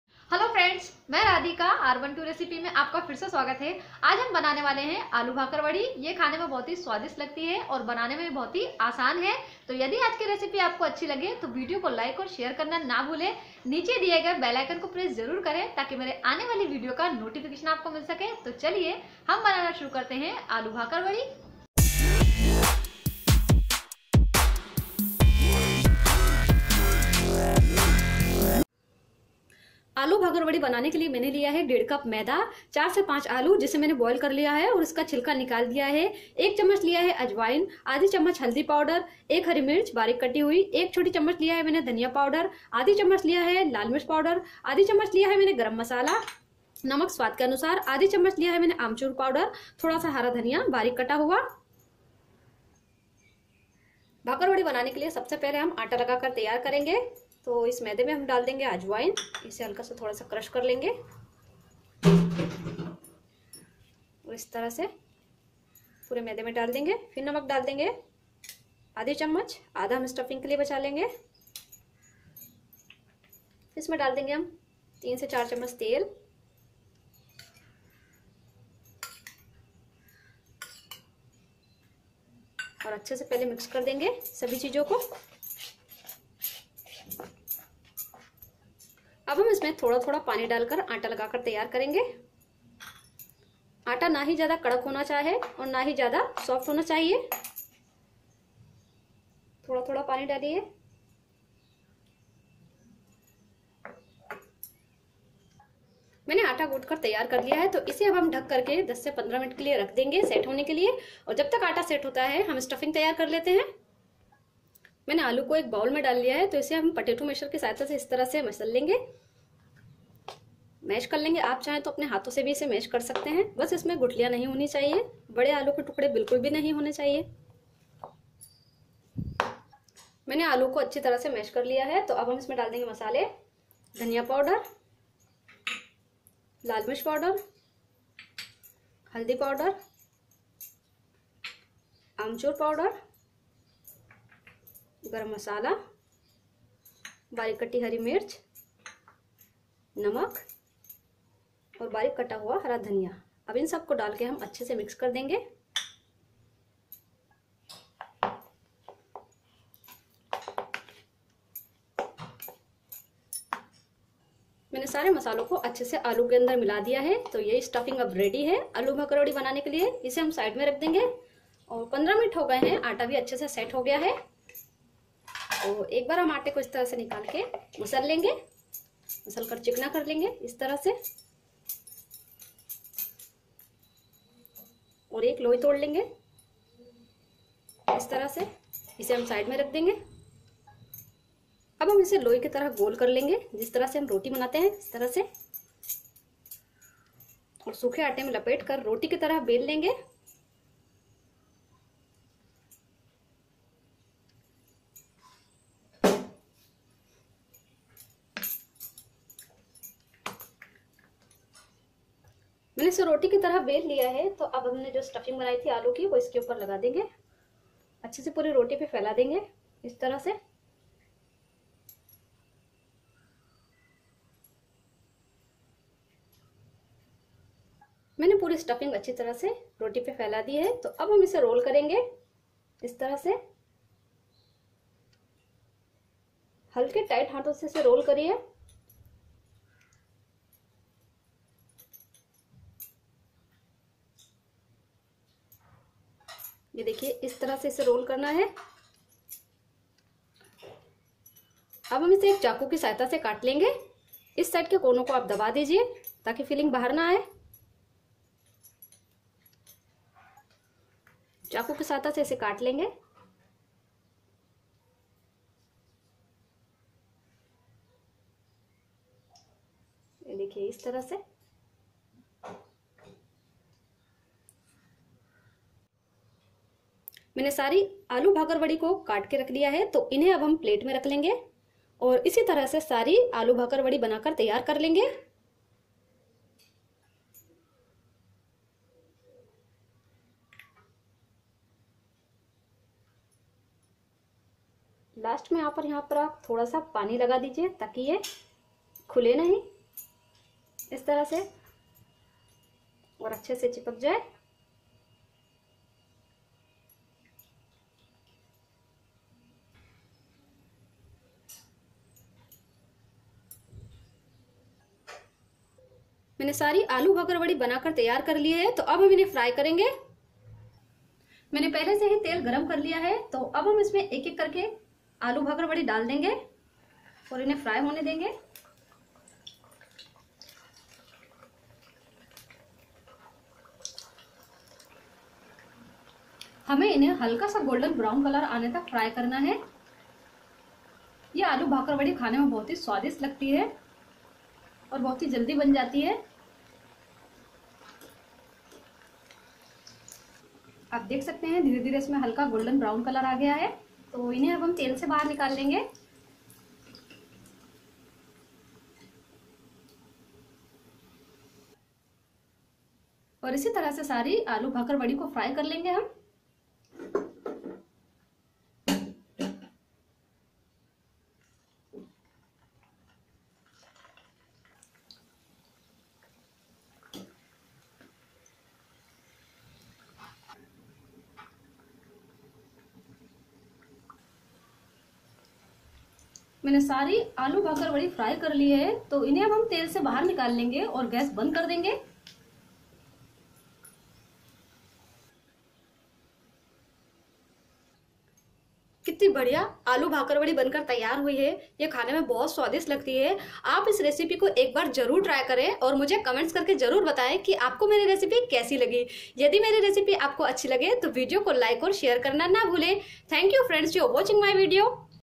मसाले वाली आलू भाकरवडी जरुर बनाये | aloo bhakarwadi | bhakarwadi recipe in hindi | breakfast recipe
bhakarwadi recipe in hindi  मसाले वाली आलू भाकरवडी बनाने के लिए हमने लिया है
1.5 कप मैदा
5 आलू बॉईल किये हुए
1/2 चम्मच अजवाइन
1/2 चम्मच हल्दी पाउडर
1 हरी मिर्च बारीक़ कटी हुई
1 छोटी चम्मच धनिया पाउडर
1/२ चम्मच लाल मिर्च पाउडर
1/2 चम्मच गरम मसाला
1/2 चम्मच आमचूर पाउडर
हरा धनिया बारीक़ कटा हुआ

aloo bhakarwadi  recipe आपको केसी लगी कमेन्ट में जरुर बताये और इस recipe को आप  breakfast recipe में बना कर खा सकते है